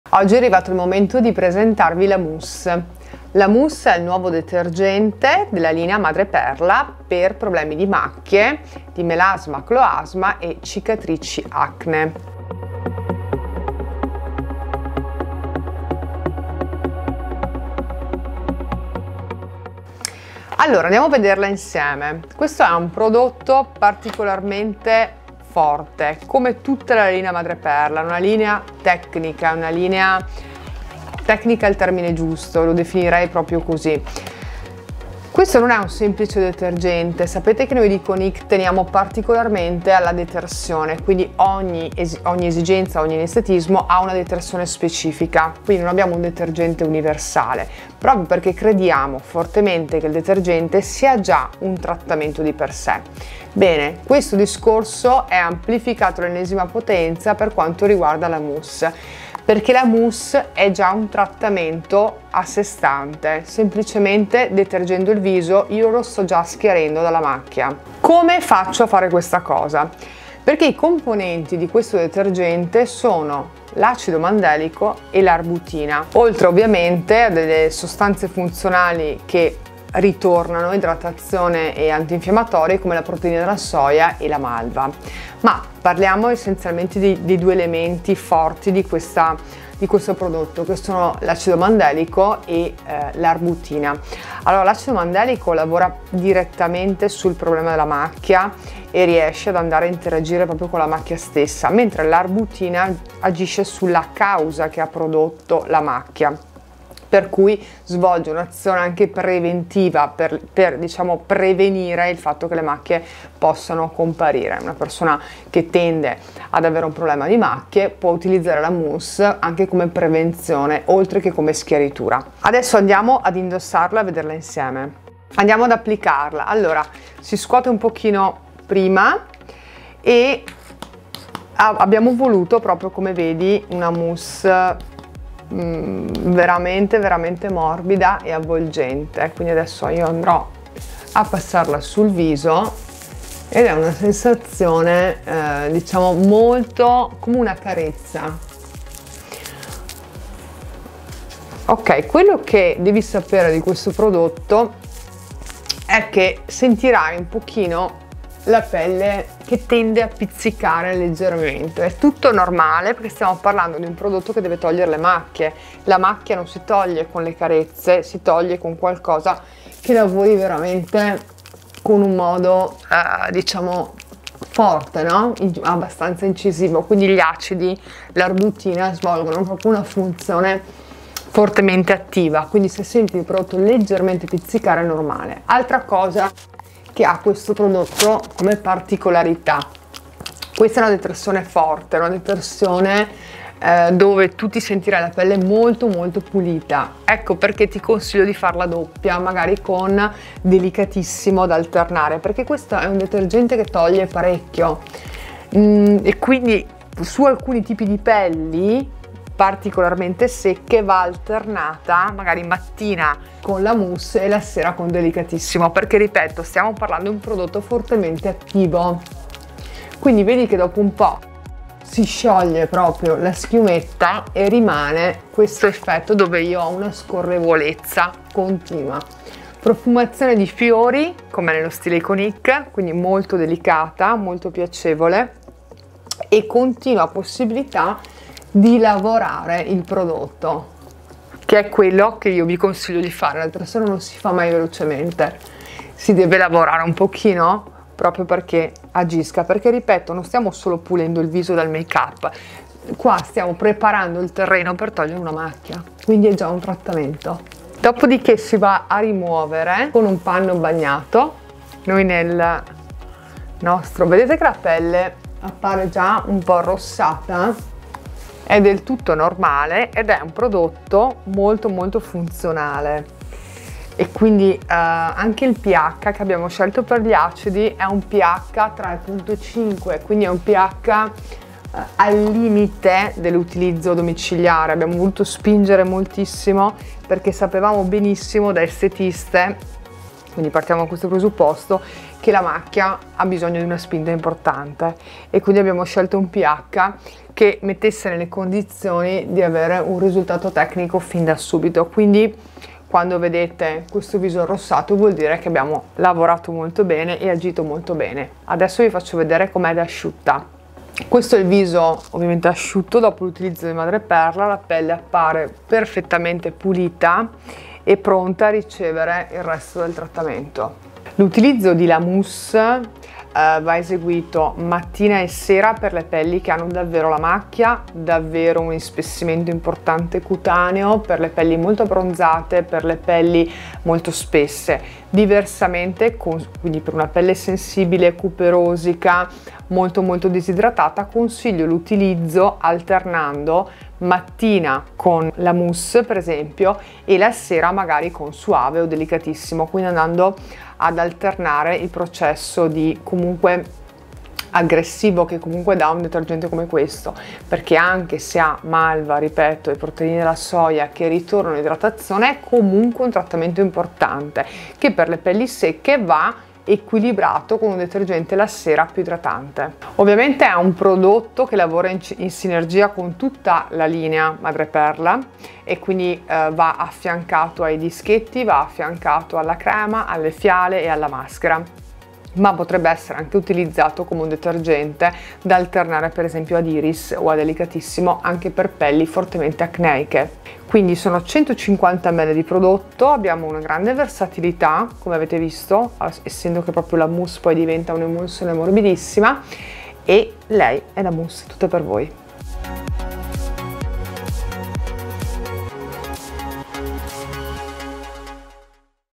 Madreperla La Mousse
Madreperla La mousse non è solo un vero detergente schiarente attivo, bensì un insieme di sostanze di origine naturale che oltre a schiarire e omogeneizzare le discromie cutanee, le previene.

La Mousse è un trattamento detergente schiarente grazie alla presenza di acido mandelico e arbutina: il primo agisce direttamente sulla discromia e sulla riparazione cutanea, la seconda agisce sull’enzima cellulare responsabile dell’eccesso della melanina, regolandola.

La texture è altamente densa e compatta, diventa cremosa mentre la massaggiamo sul viso ed è perfetta in caso di macchie di melanina, macchie post acne, melasma e cloasma.

È un detergente altamente bilanciato e completo, non solo per la sua azione schiarente già in fase di pulizia della pelle, ma anche per i suoi fattori antinfiammatori idratanti e astringenti che aggiungono ulteriori finalità fino a far diventare il prodotto un “trattamento”.

Il contenuto è privo di alcool e solventi potenzialmente irritanti per la pelle.

La Mousse inoltre regala nell’immediato leggerezza alla pelle grazie alla sua azione esfoliante e levigante, liberandola da cellule morte e impurità.